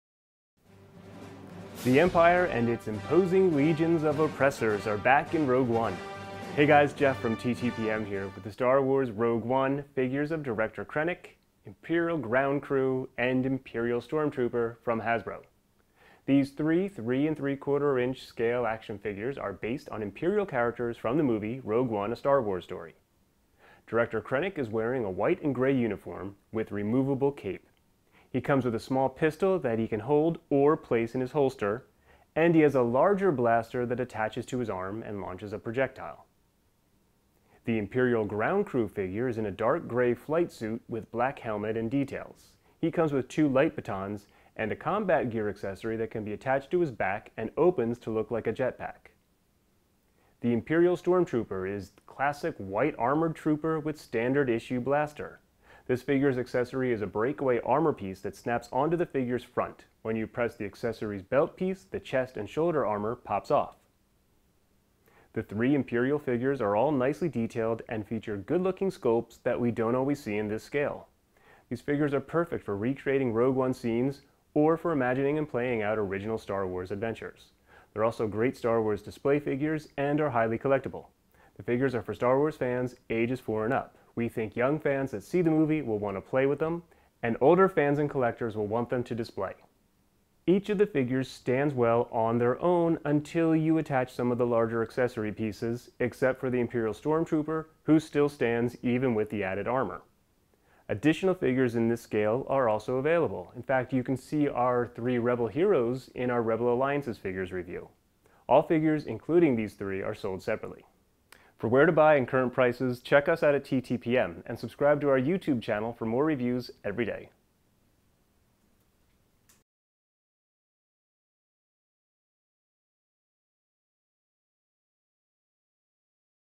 Star Wars Rogue One 3 3/4 inch Imperial Figures from Hasbro
STAR WARS ROGUE ONE 3.75-INCH ACTION FIGURES REVIEW from TTPM! Hasbro’s Rogue One figure collection includes Director Krennic, Imperial Ground Crew, and Imperial Stormtrooper. See all three unboxed figures in this TTPM full video review.  For full review and shopping info

Product Info: The Galactic Empire, and its imposing legions of oppressors are back in control in the Star Wars movie, Rogue One. The Star Wars Rogue One 3.75-inch action figures of Director Krennic, Imperial Ground Crew, and Imperial Stormtrooper, from Hasbro, are figures of the Imperial characters in Rogue One: A Star Wars Story.